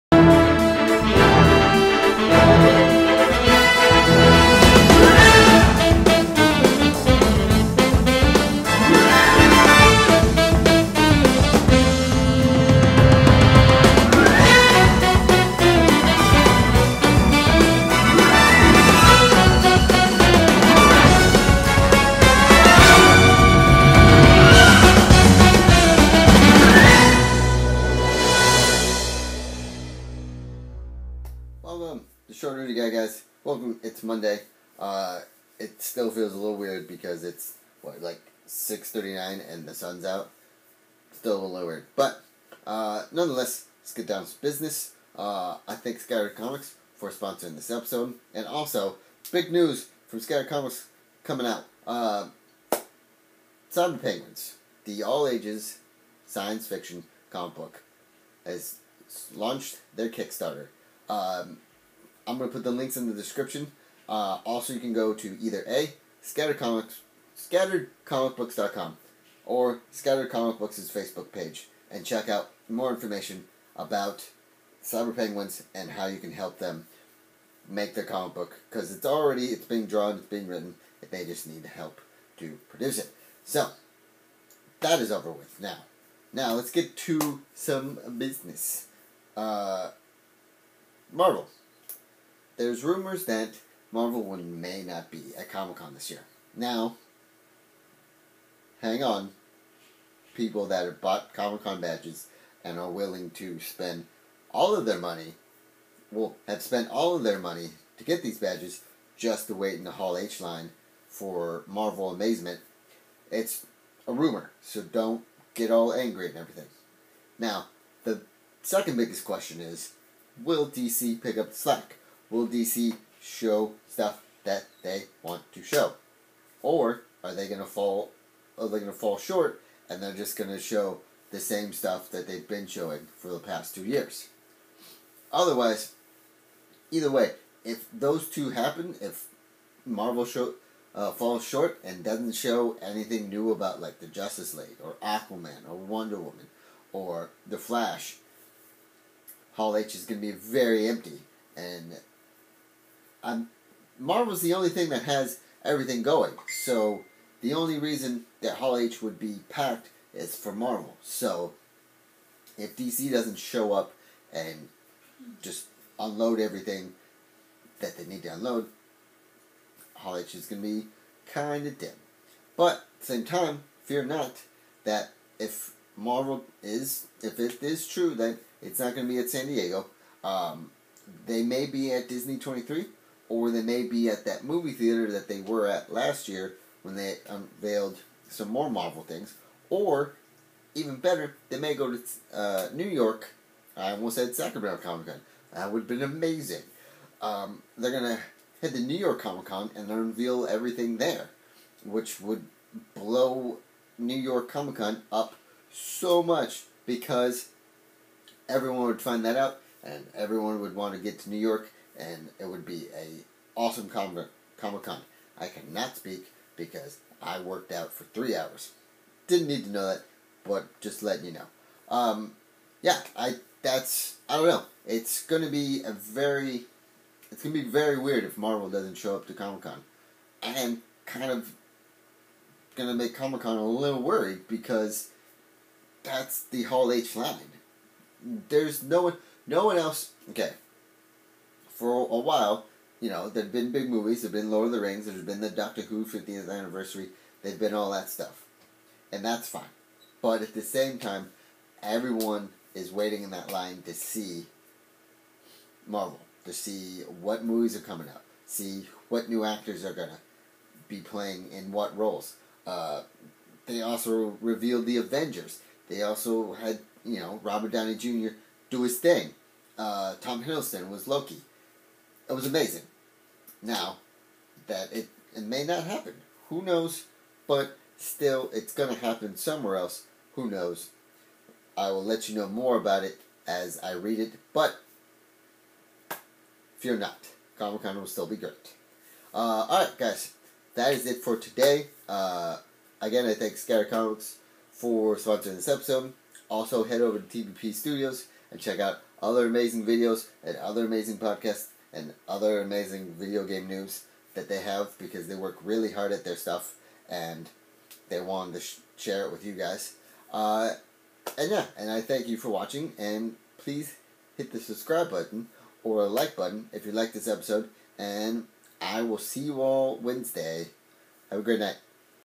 No Marvel in SDCC?
Welcome to the being of the week. Today on short nerdy guy I talk about  the new Kickstarter that Scattered Comics is doing for cyborg penguin. Also, there is roomier hat Marvel well no be heading to Comic-con this year. Which mean will Dc comic  step it up and blow the roof off of Hall H or fall short and just show more Batman v Superman stuff?